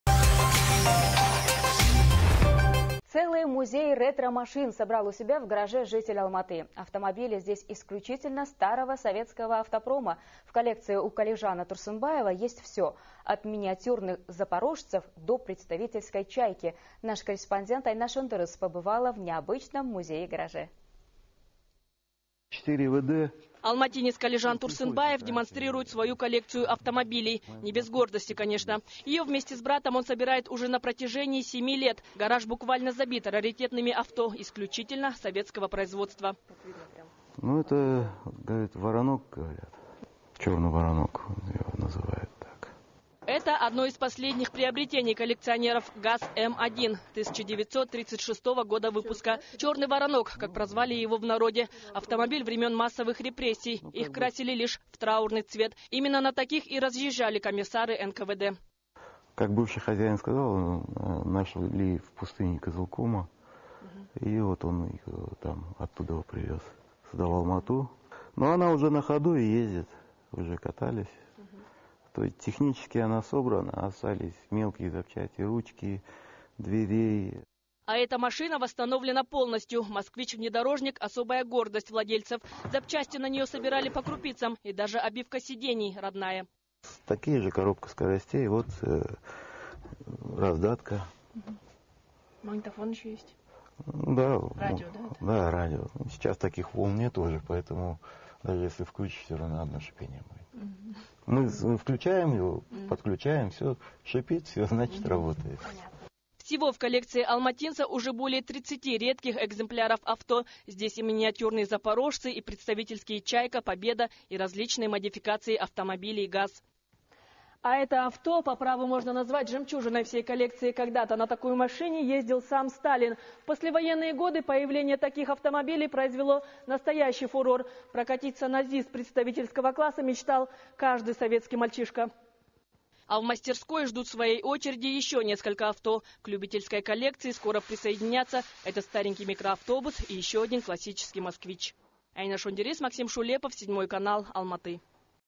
Более 30 раритетных советских авто собрал в своей коллекции житель Алматы
Целый музей ретро-машин собрал у себя в гараже житель Алматы! Автомобили здесь исключительно старого советского автопрома. В коллекции у Калижана Турсынбаева есть всё - от миниатюрных запорожцев до представительской «Чайки».